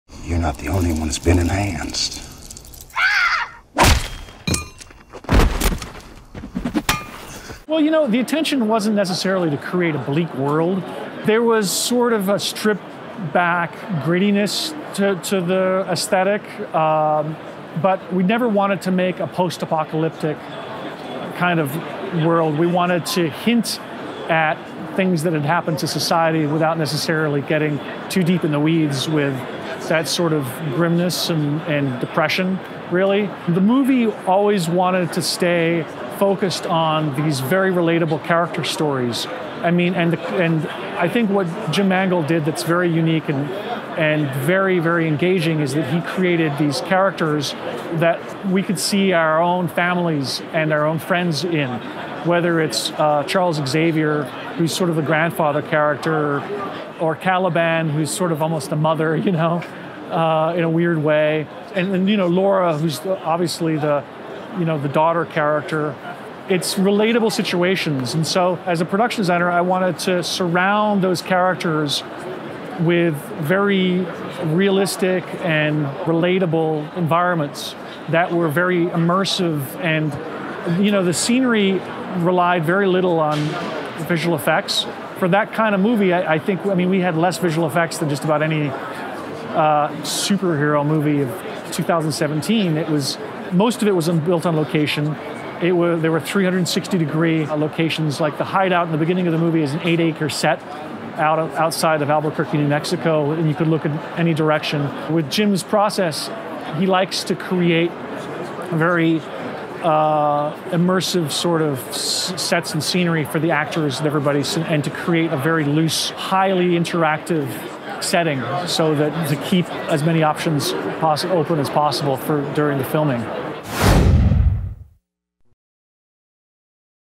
Logan’s production designer on bringing a post-apocalyptic world to life
Logan surprised us earlier this year by being an uber-violent but measured character drama, a notable departure from most modern-day superhero flicks. The film paints a vivid picture of a somewhat post-apocalyptic United States where the majority of the country’s mutants have been wiped out. Helping bring this vision to life is production designer François Audouy. After a production designers’ autograph session at this year’s Comic-Con, we sat down with Audouy to see how he approached creating the world we see in Logan.